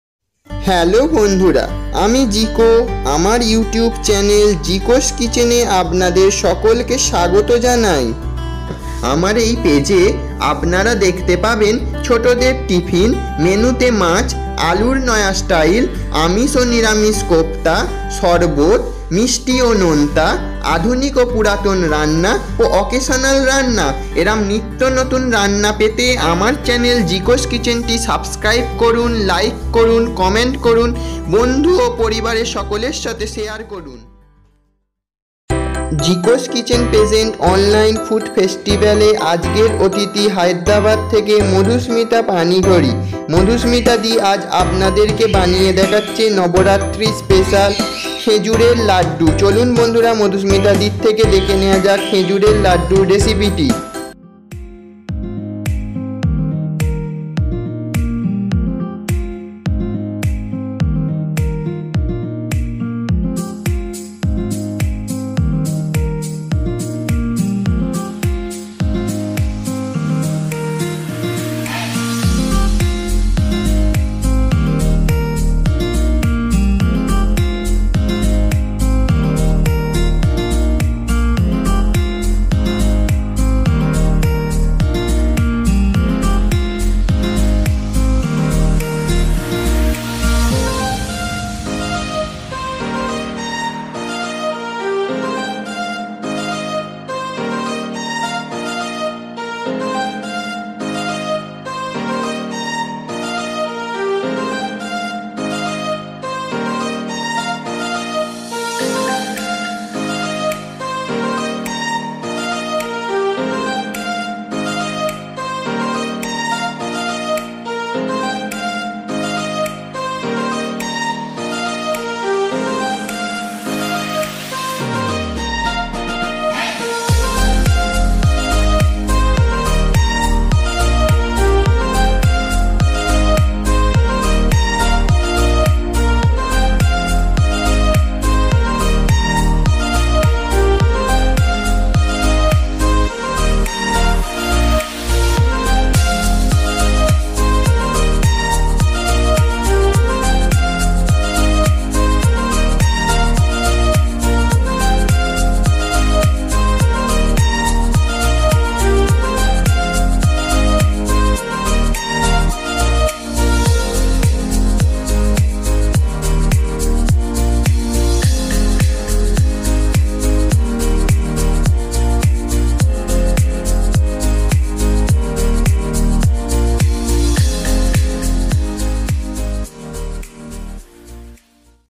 NABARATRI SPECIAL FRUIT AND NUTS KHAJOOR LADOO BY MADUSMITA PANIGRAHI .
I wish the blessing of Maa Durga fills your life with happiness and prosperity. Happy Durga Puja 2020.

Name :MADHUSMITAPANIGRAHI

Recipe

FROM: HYDERABAD

YOUTUBE CHANNEL : THE FOOD EXPERIMENT.

শুনে মতামত জানার অপেক্ষায় রইলাম,ভালো লাগলে like ও share করার অনুরোধ রইল।